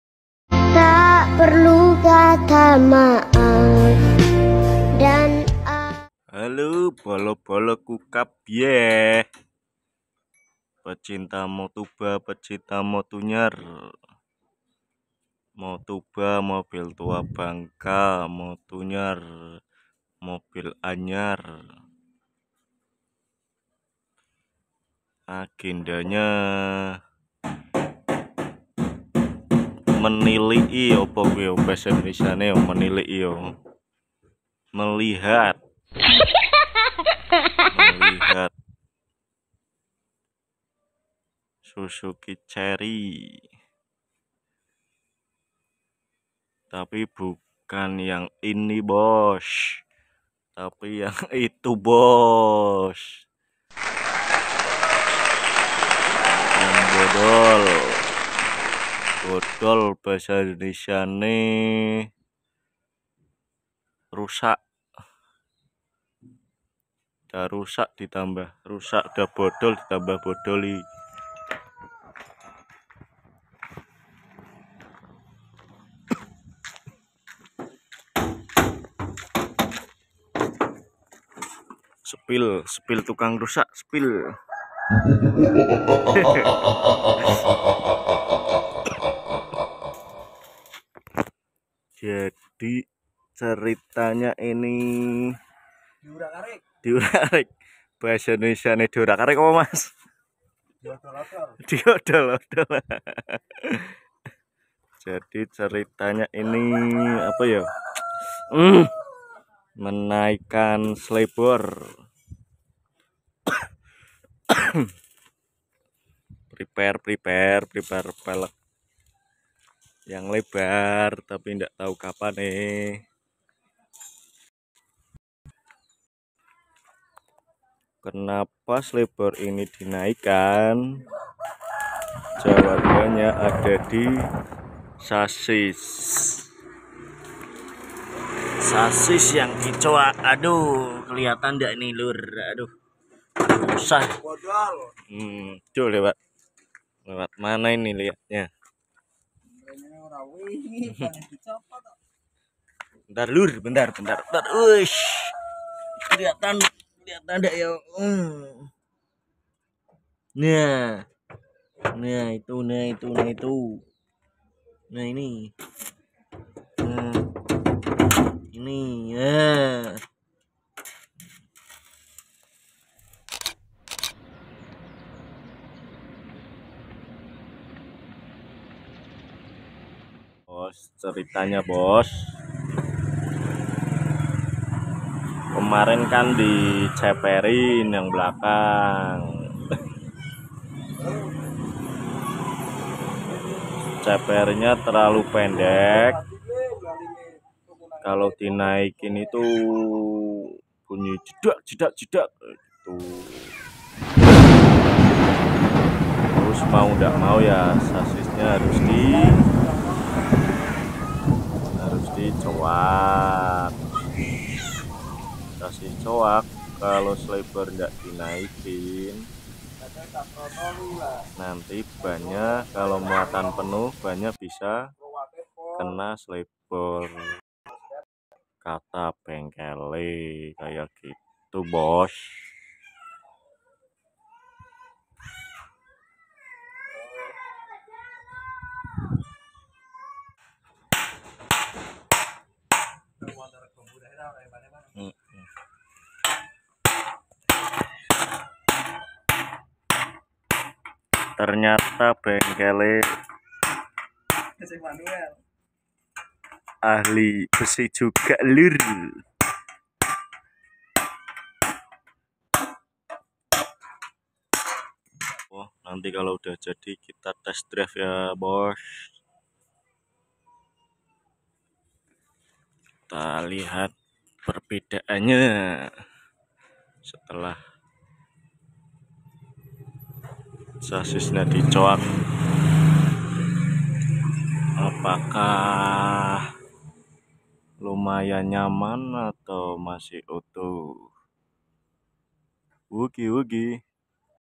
CARRY 1.0 buoshok,. coak casis dan naikin slebor belakang
ceper tp nyaman gimana ya caranya